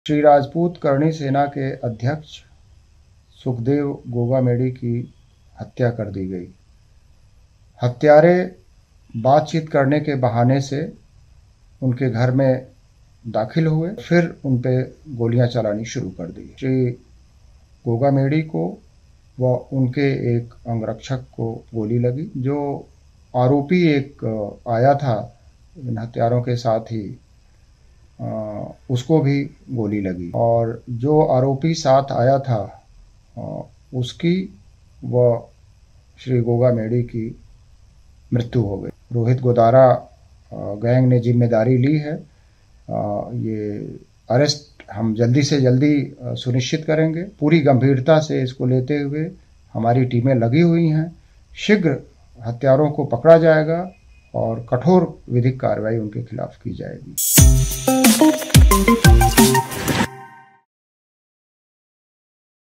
Sukhdev Singh Gogamedi News: DGP ने बता दिया कौन है करणी सेना अध्यक्ष का हत्यारा ? ABP LIVE
News: राजस्थान के जयपुर में श्री राष्ट्रीय राजपूत करणी सेना के प्रमुख सुखदेव सिंह गोगामेड़ी को गोली मारने वाले दोनों आरोपियों की पहचान हो गई है. एक आरोपी का  नाम रोहित राठौर है. जो कि नागौर के मकराना का रहने वाला है. वहीं दूसरे का नाम नितिन फौजी है. वो हरियाणा के महेंद्रगढ़ का रहने वाला है. फिलहाल दोनों फरार हैं. दोनों ने मिलकर सुखदेव सिंह गोगामेड़ी को गोली मारकर मौत के घाट उतार दिया था.